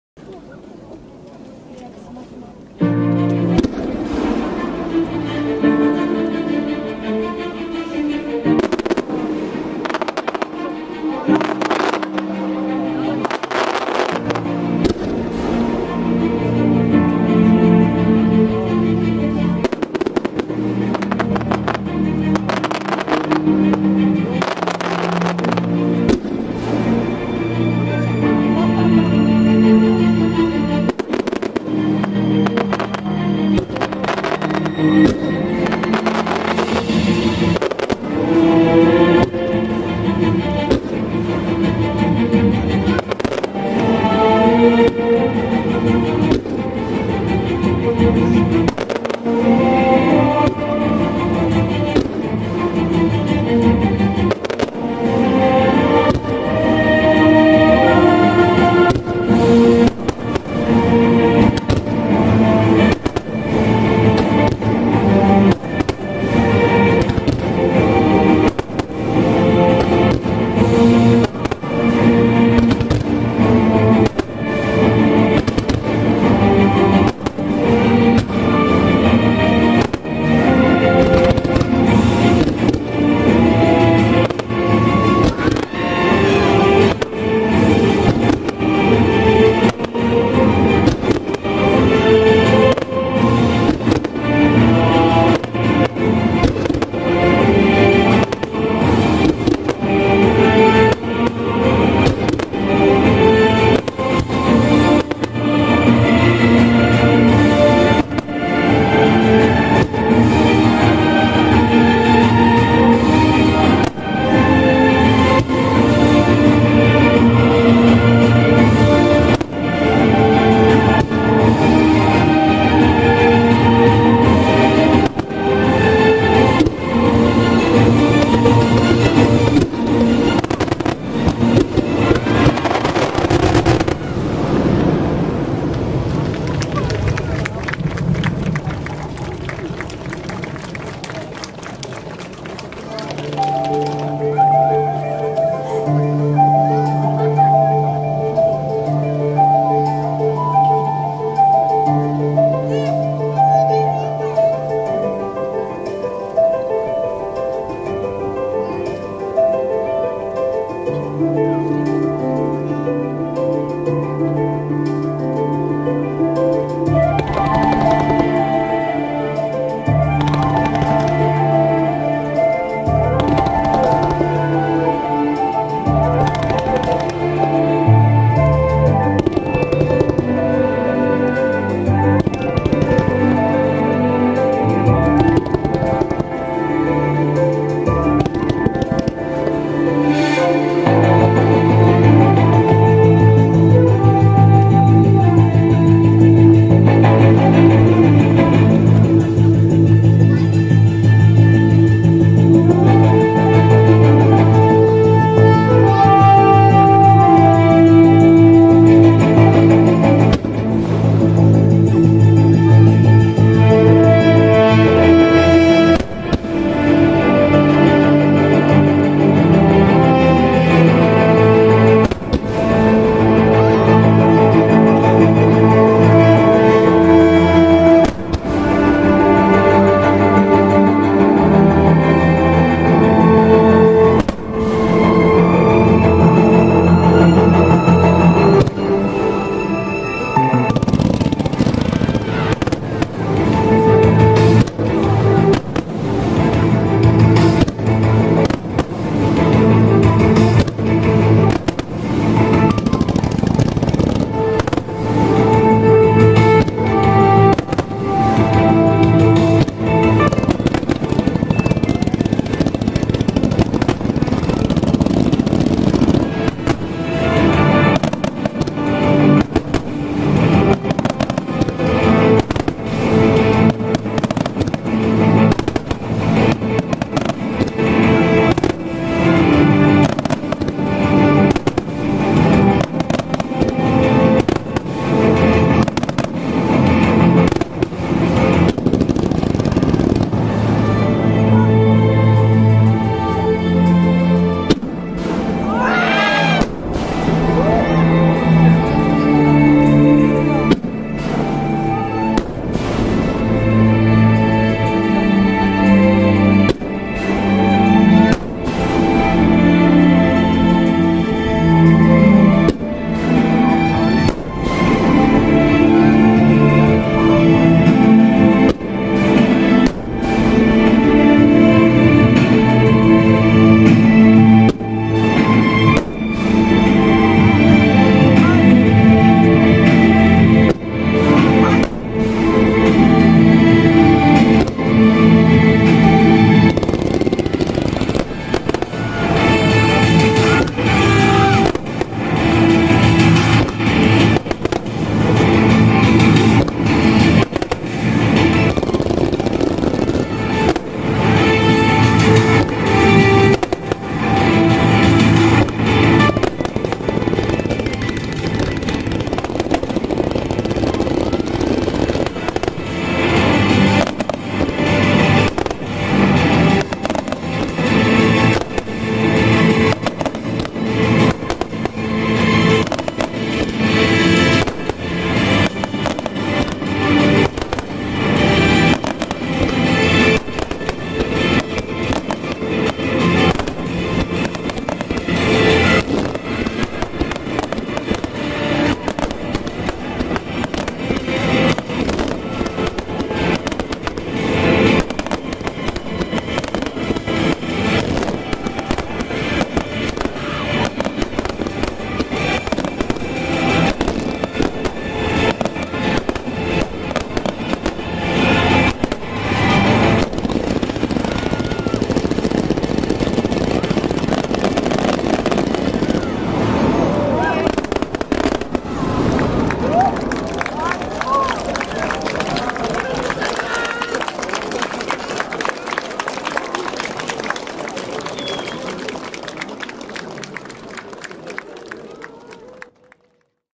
2021年7月10日 能代港疫病退散花火 第２部 ミュージックワイドスターマイン
2021年7月10日、能代港で実施された疫病退散花火（サプライズ企画）のうち、第２部ミュージックワイドスターマインの動画です。

hiramuさん風に編集してみたので、画面いっぱい、４Kでお楽しみください。
※彩度アゲアゲ、和火強調処理してま～す。